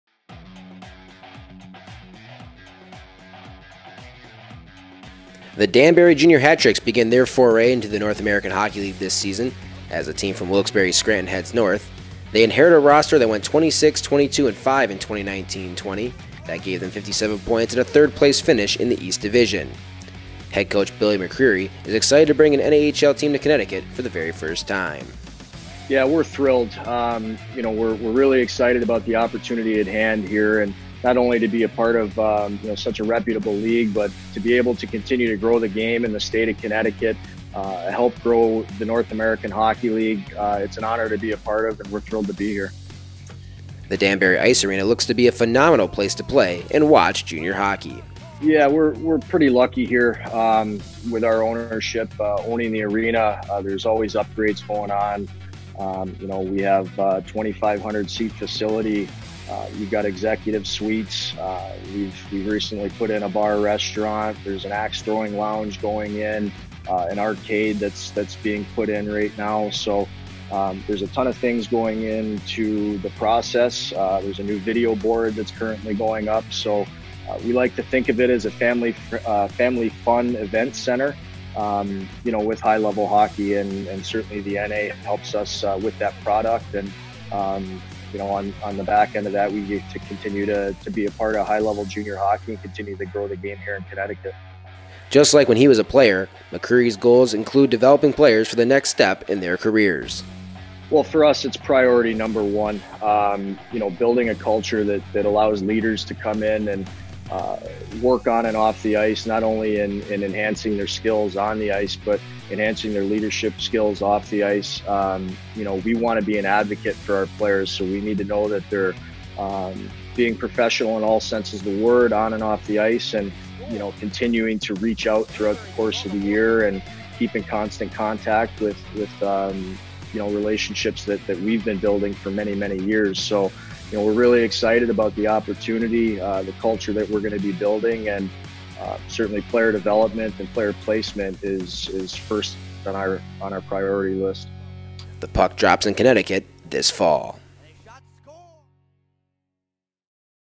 NA Now - Danbury
The Danbury Jr. Hat Tricks prepare for their first season in the NAHL!